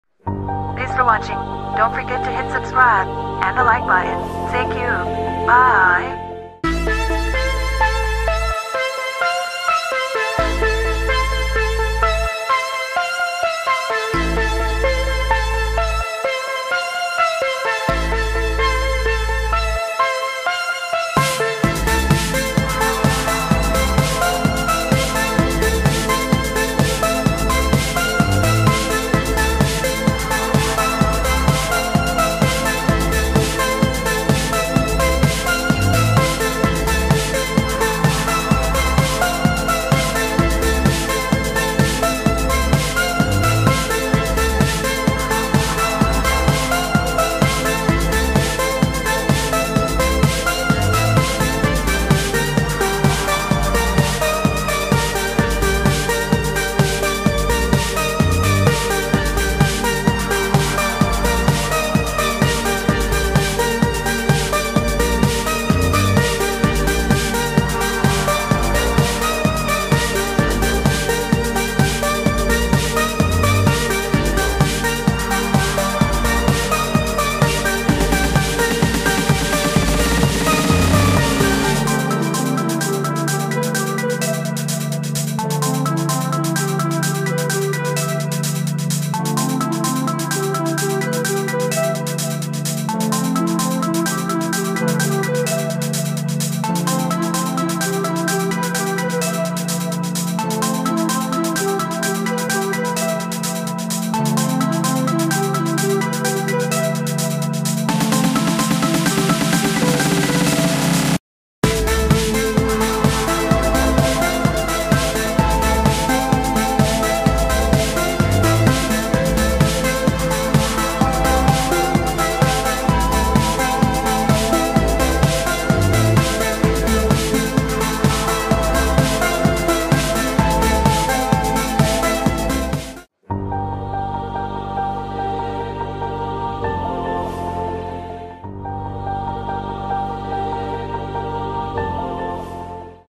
Timelapse planet/galaxy acrylic painting!! (Unkown Civilization)
Happy with this one... let me know your thoughts on the planets really focusing on getting perfect circles.